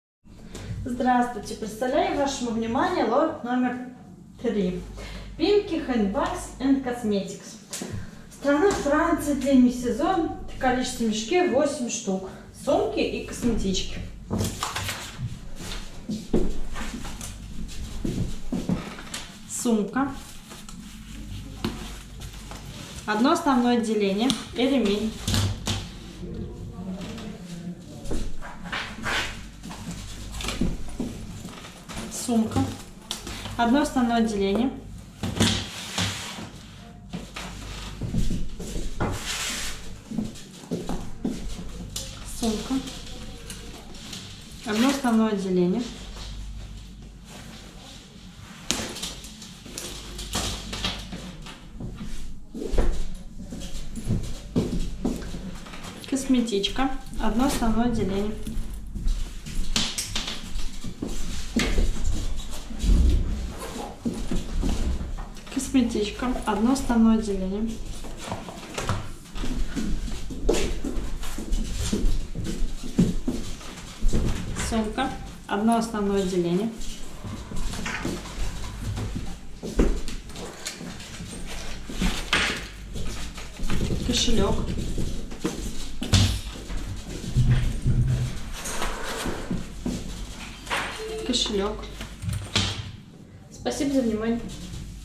PIMKIE HANDBAGS&COSMETICS *3, сток одежда оптом
PIMKIE HANDBAGS&COSMETICS (лот №3)
Сумки и косметички (сток)
Страна: Франция
Демисезон
Цена за ед.: 3.67 евро
Цена за мешок: 29.36 евро
Кол-во в мешке,шт.: 8